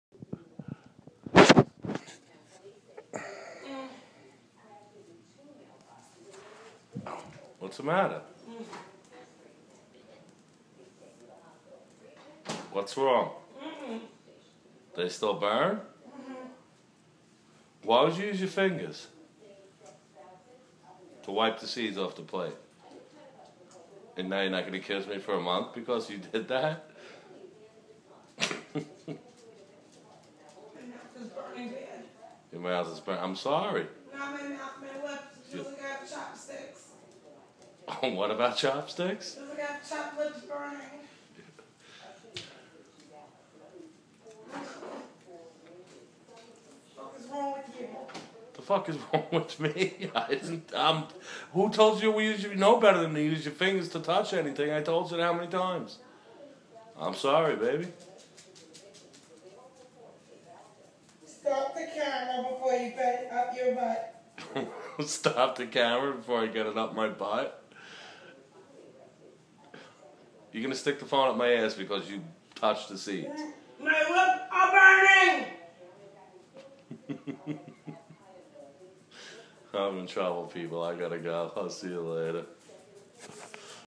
Naga Viper Revenge
Here's what happened to sharon after I gave her the knife and plate that I cut the viper with...